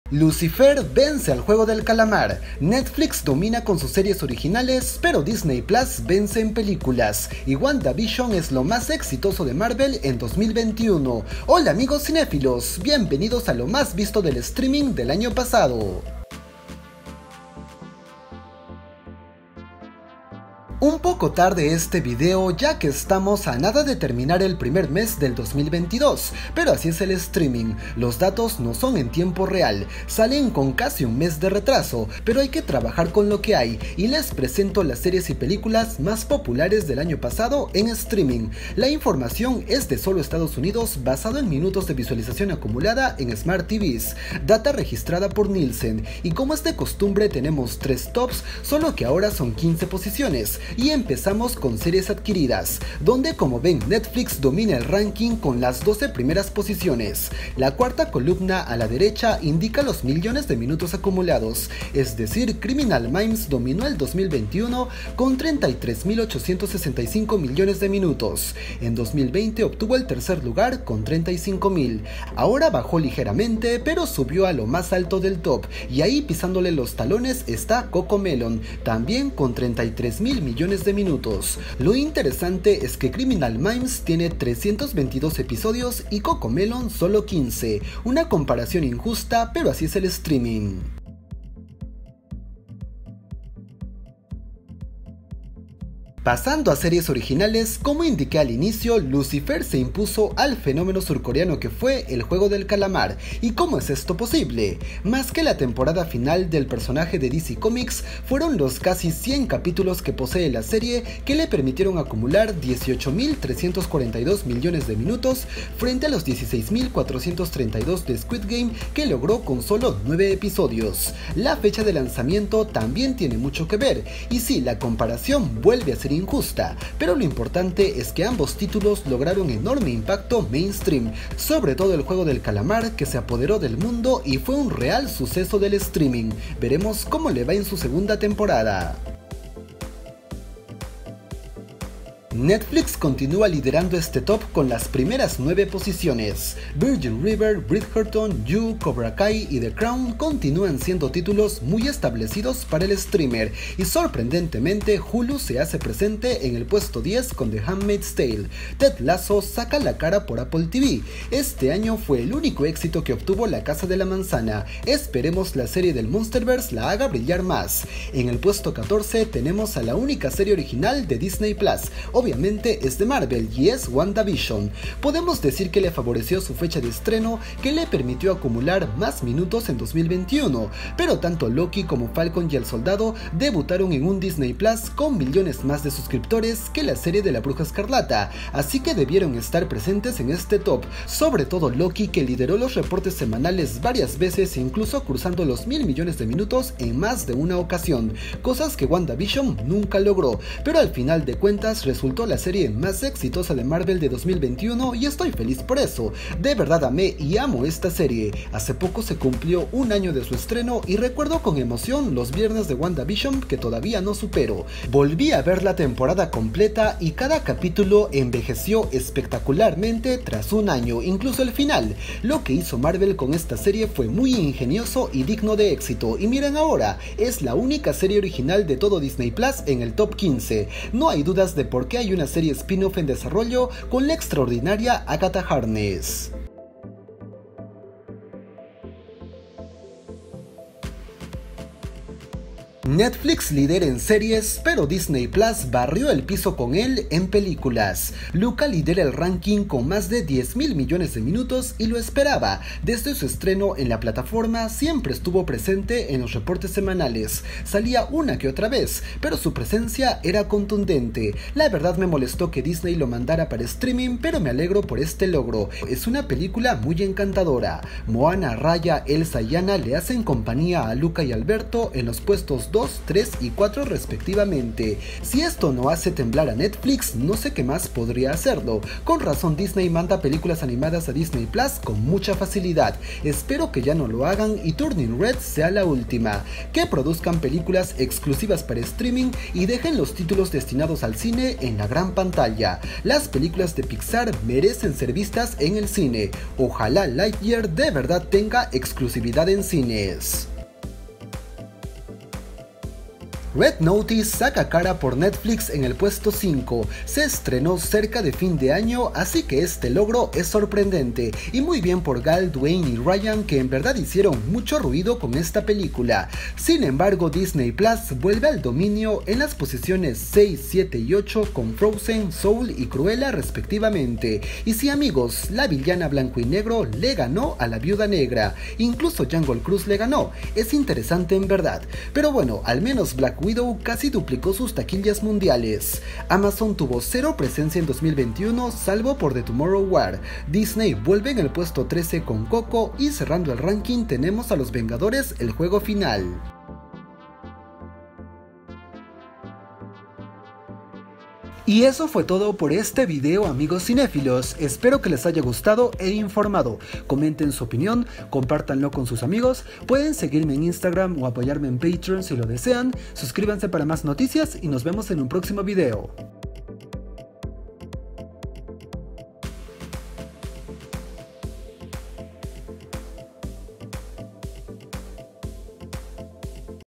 Lucifer vence al Juego del Calamar, WandaVision es la mas exitosa de Marvel, Luca la cinta mas vista
Descubre las series y películas mas vistas en streaming de 2021.